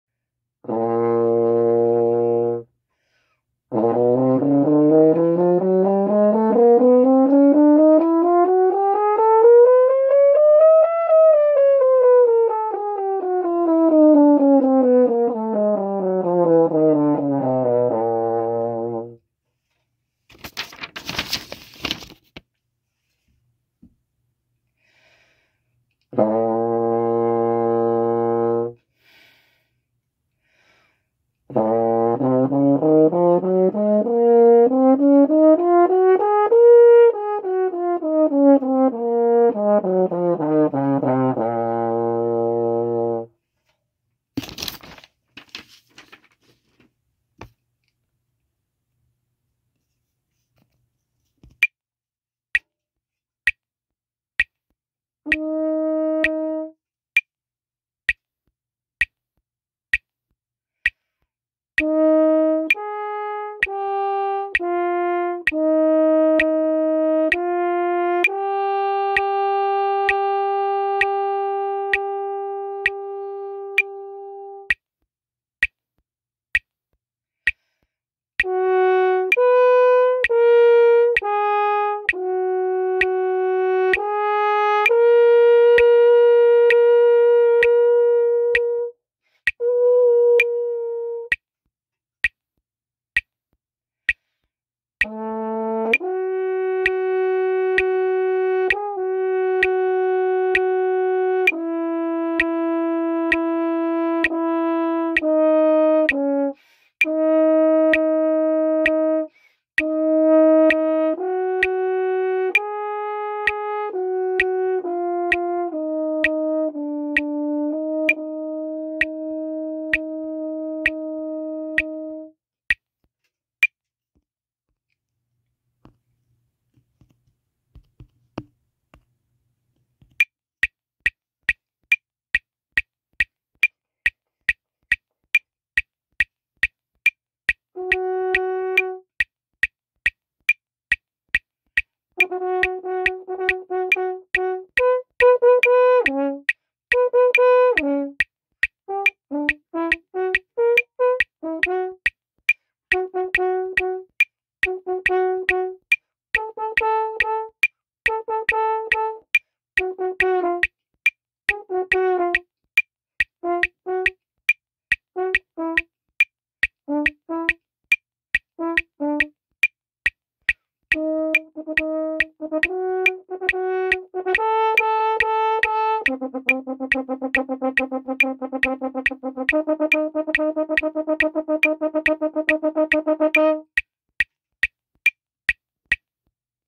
Ysanni Core_French Horn
French Horn Audition video for CMU's AUO for Ysanni Core